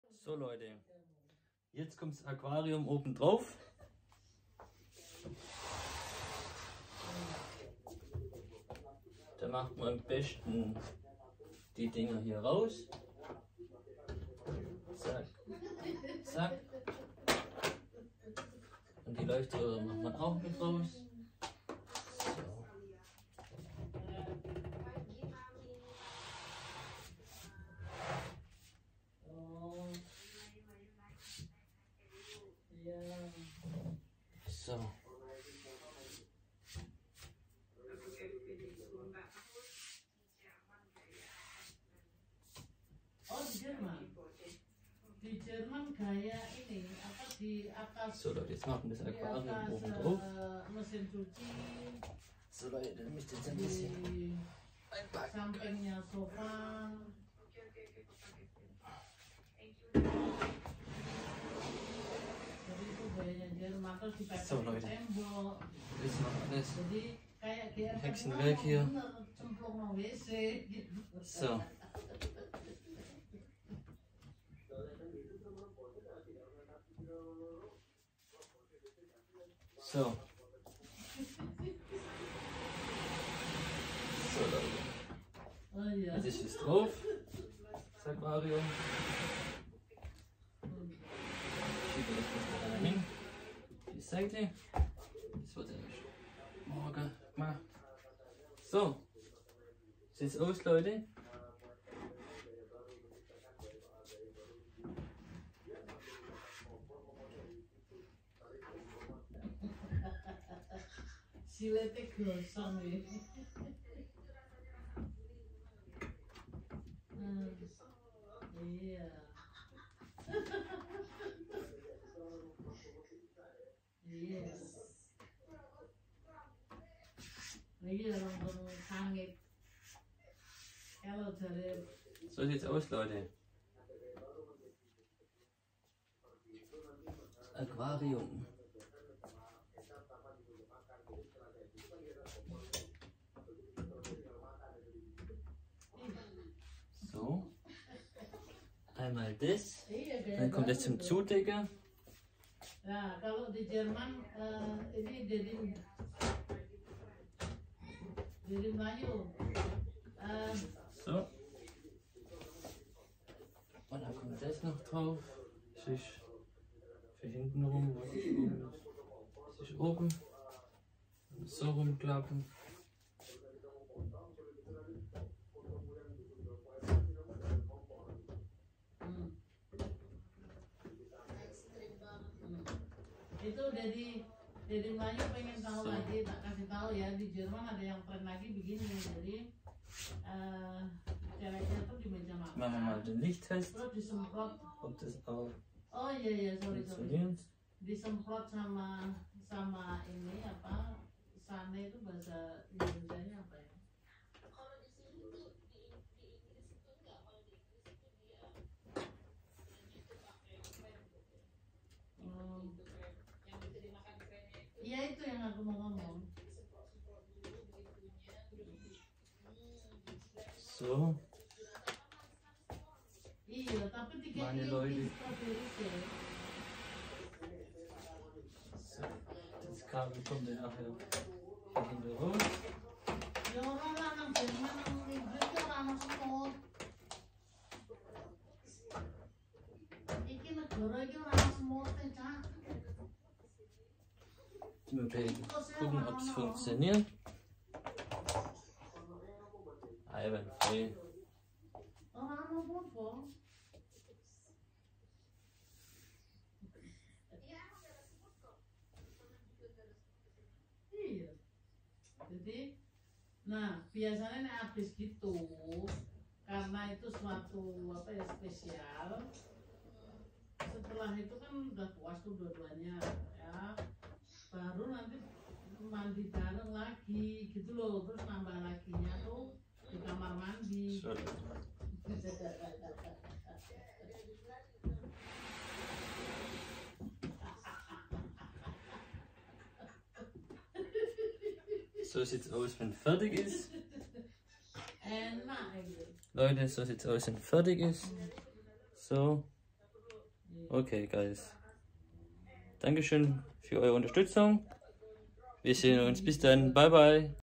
180 Liter Aquarium Aufbau 👍🏽🐠🐟
In diesem Video geht es um mein Aquarium dass ich aufgebaut habe.

Es ist ein 180 Liter Aquarium ✌🏽👍🏽😉